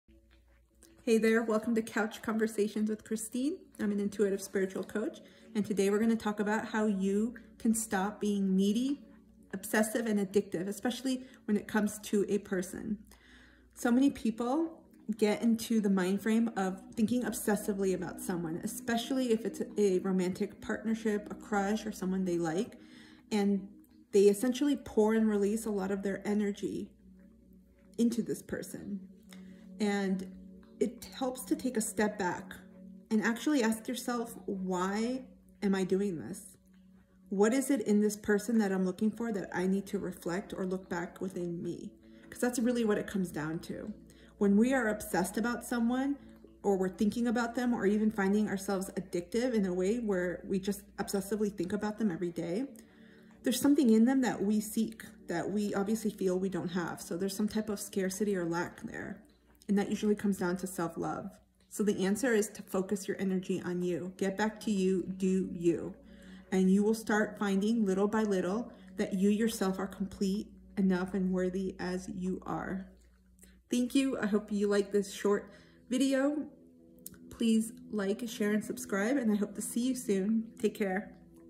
How to Stop Being Needy!
Have you ever found yourself becoming obsessive or addicted to someone? Like you can't stop thinking about them? Whatever the reason is - and know this doesn't have to be a romantic relationship - know you can quiet this neediness! Being needy is not good or bad, it just is. But when we take a deeper look at why we do this, we can realize what it is we're truly after.